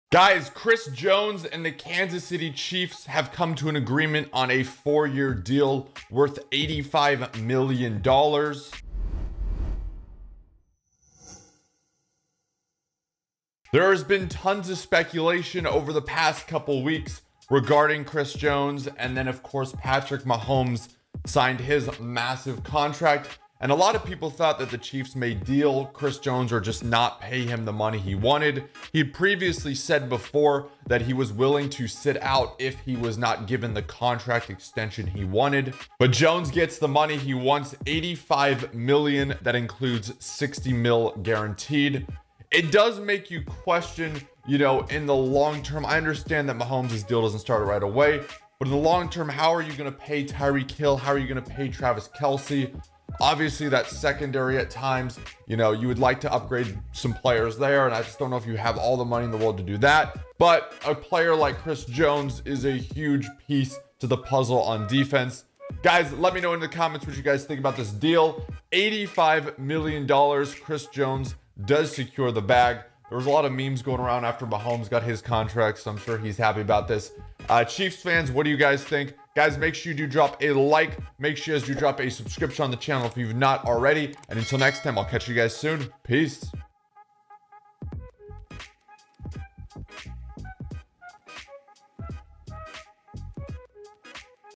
Chris Jones SIGNS a $85 Million Deal with the Kansas City Chiefs!
Kansas City Chiefs defensive tackle Chris Jones has signed a 4 year 85 million dollar extension with the Chiefs!